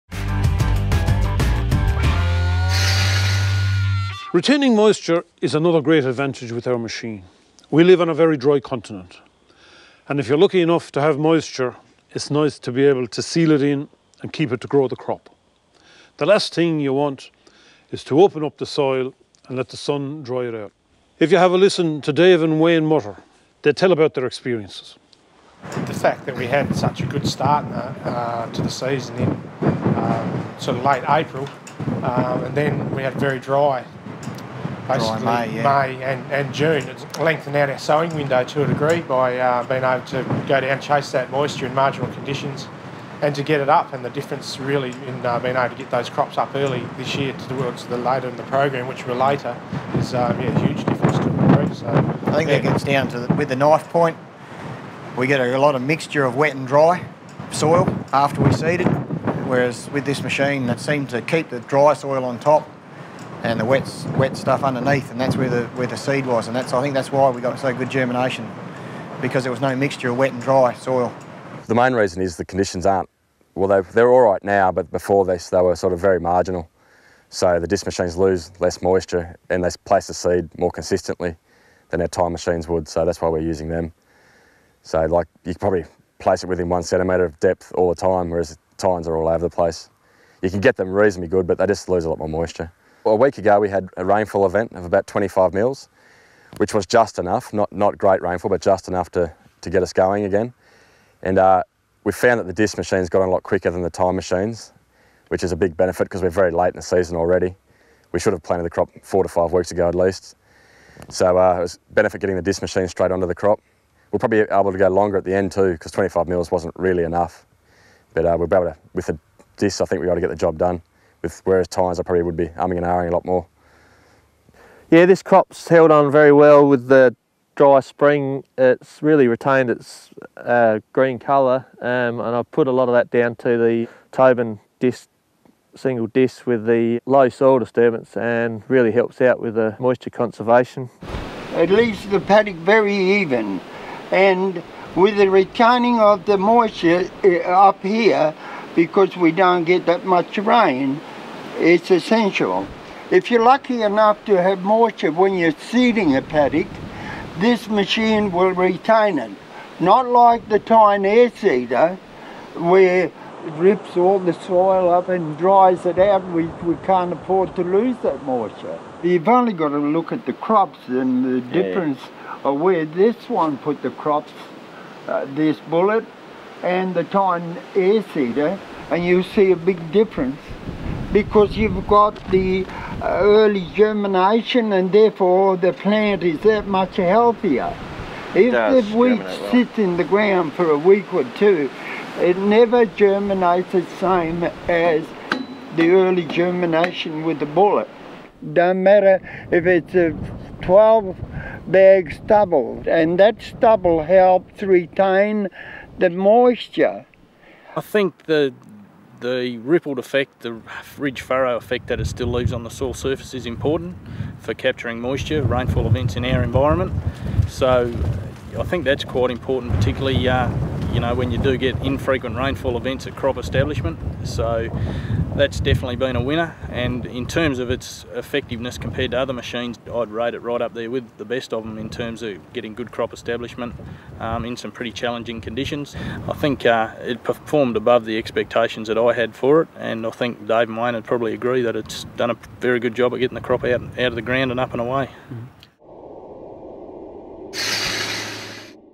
No Till - 05 Retain Moisture - Tobin No-Till - Disc Drill Seeding
In marginal conditions is nice to be able to keep the moisture you have in a paddock and use it to grow the crop. This video detail how the Tobin performs in marginal conditions.